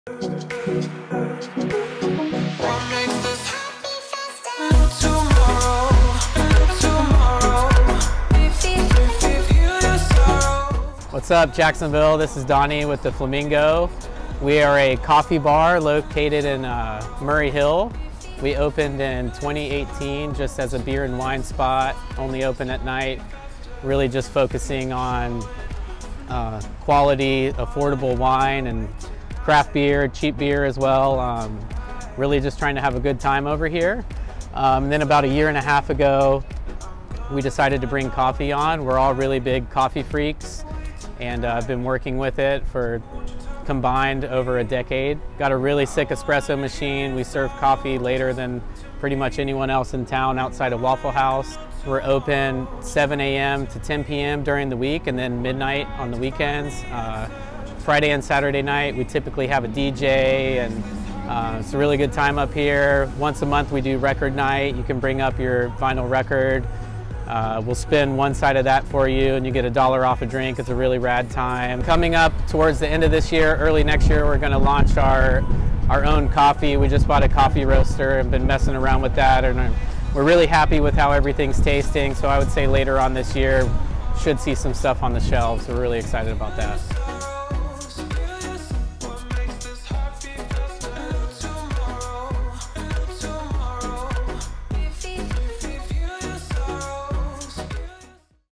The Flamingo | Murray Hill Coffee Shops
The Flamingo in Murray Hill is a must try when it comes to Jacksonville coffee shops! We talked to Donnie (the flamingo owner) about his coffee shop, its history, and the full rundown when it comes to the flamingo! Check them out below as well as our coffee shops and Murray Hill Guides!